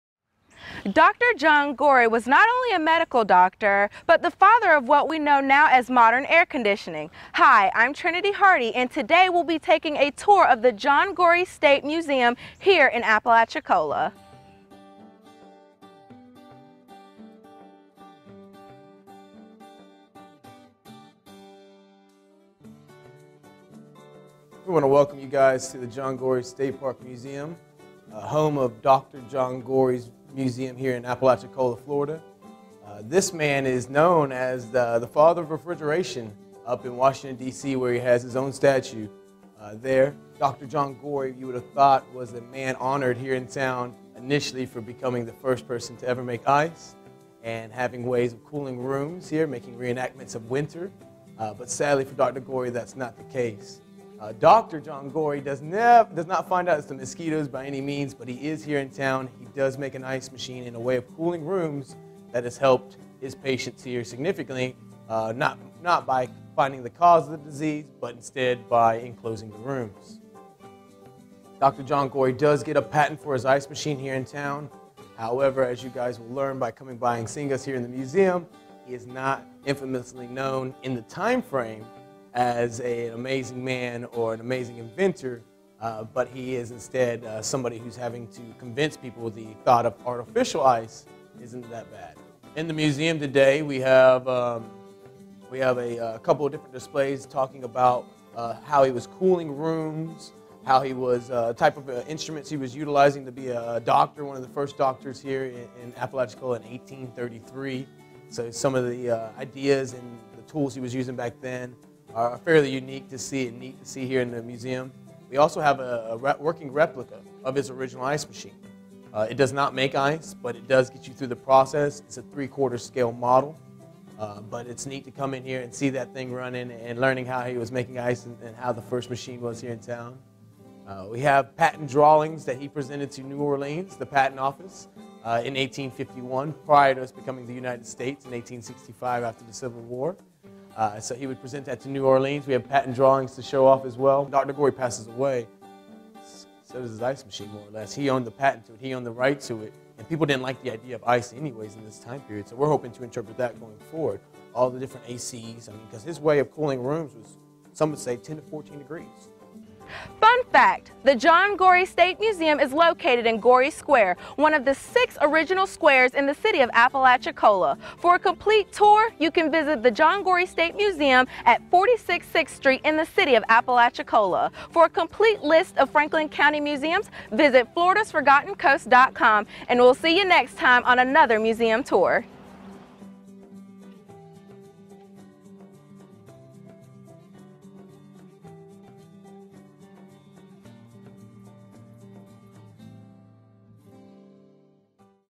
John Gorrie Museum
Take a closer look inside the John Gorrie Museum located in Apalachicola, Florida with host Trinity Hardy.